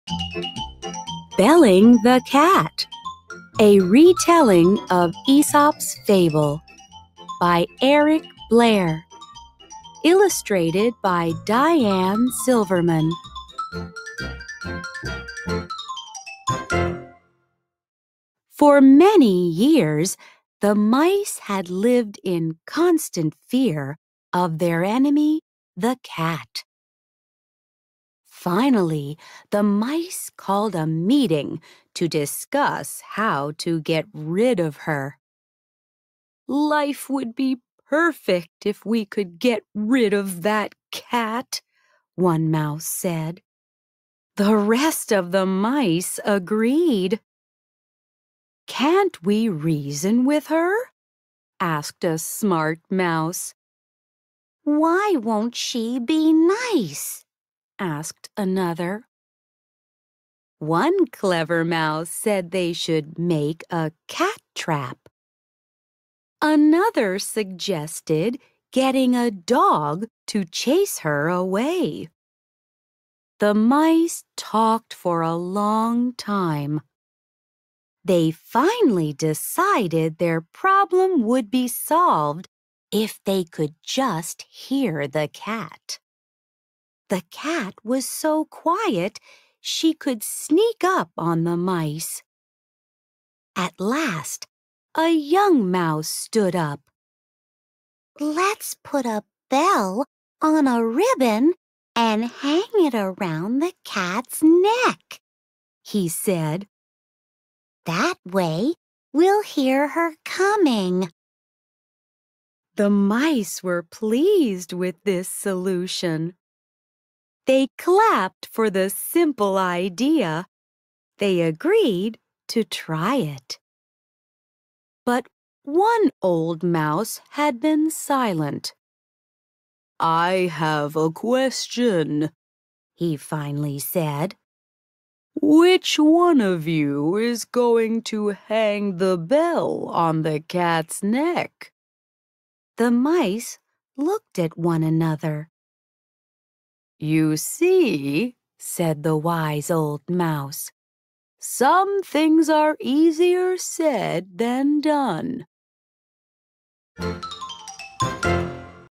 Belling the Cat
This is Aesop's fable about Who will Belling the Cat